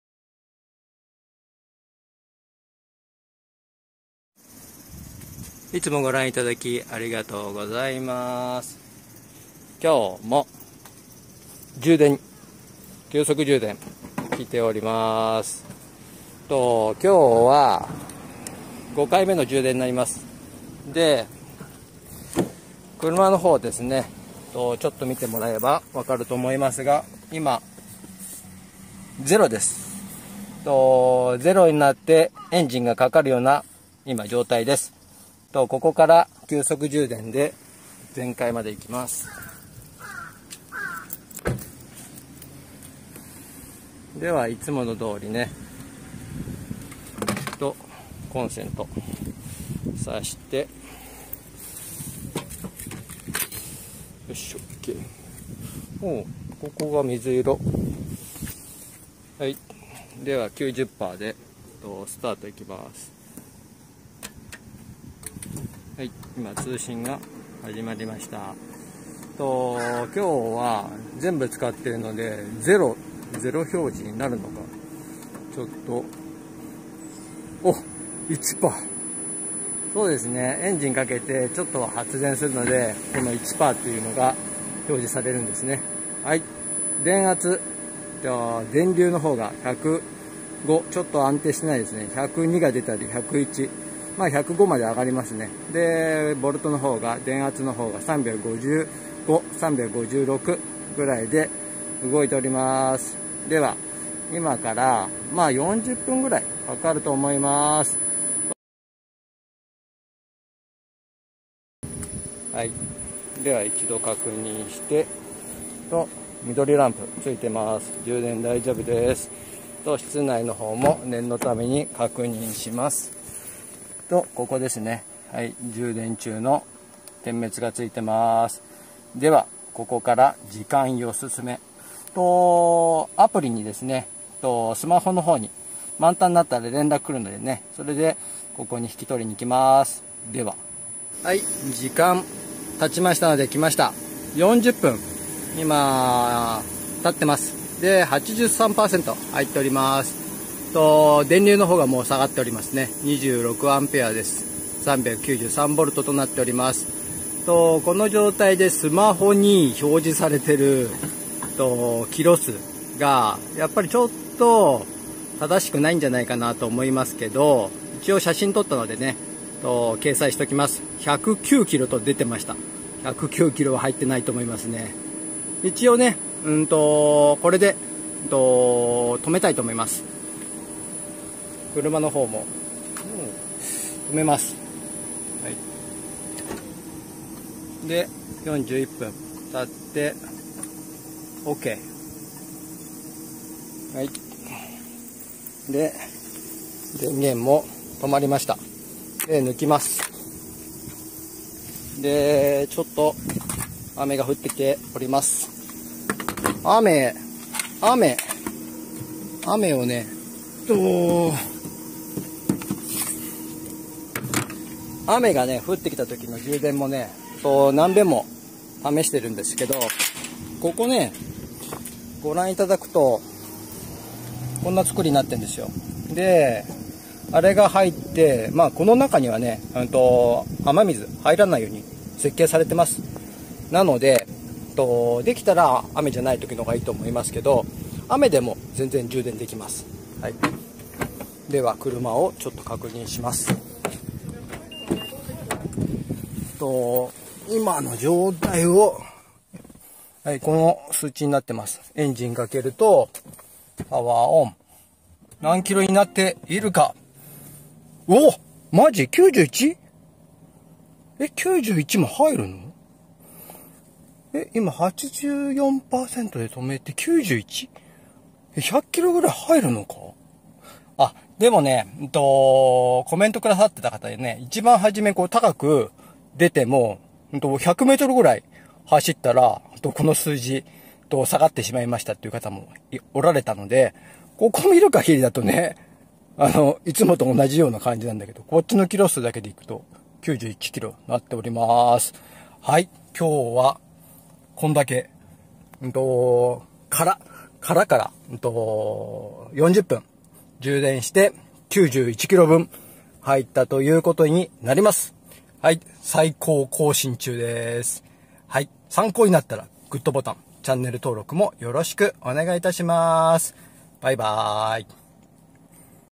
【お得にカーライフ】『三菱 新型アウトランダーPHEV』急速充電量がカタログ値（83km）を超過した！過去最高をさらに更新！
５回目の急速充電をしてきました。
そこで、充電量が『91km分』と過去最高をさらに更新しました。
・・・Pグレードのカタログ値は、83kmを超過した！
※今回は、途中で停止したので、95kmぐらいは充電可能なのか？）

☆目標として、『登録者1,000人』を目指しております。少しでも皆さんのお役に立てれば光栄です。これからも、動画配信を継続していきますので、よろしくお願いいたします。